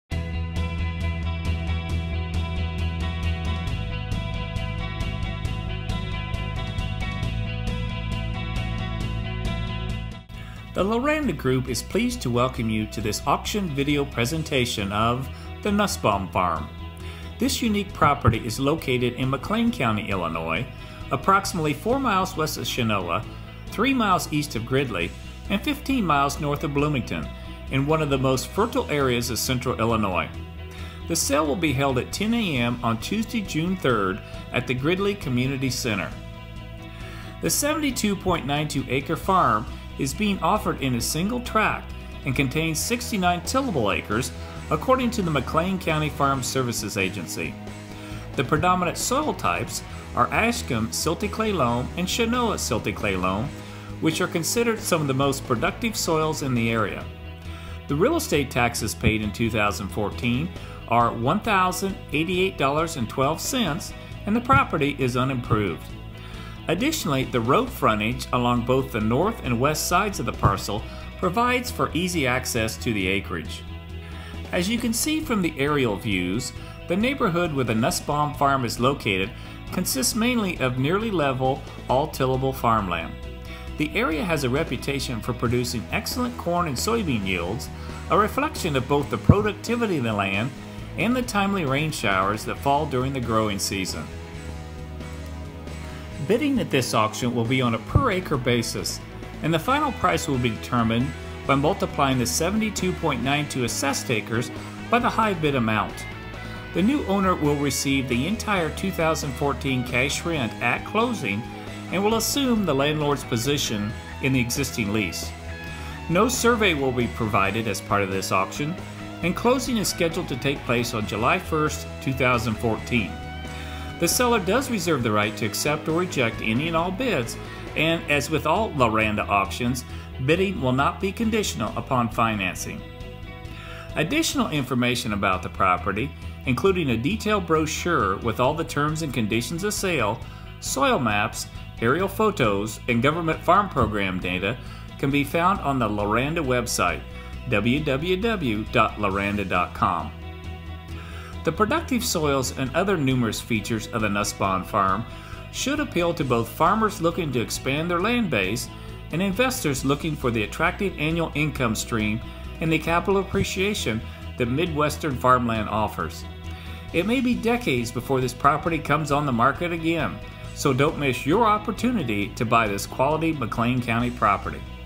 McLean Co., IL 72 Acre (Nussbaum Farm) Land Auction Promotional Video by The Loranda Group
This Central Illinois farm land auction tract is located 3 miles E of Gridley, 4 miles W of Chenoa, or 15 miles N of Bloomington, will appeal to both farmers and investors.  The 72 acres is nearly all tillable and features quality soils.  The successful buyer will receive the full 2014 cash rent payment of $17,275 at closing

PUBLIC AUCTION - June 3 at 10:00 AM; 72 +/- Acres in McLean Co., IL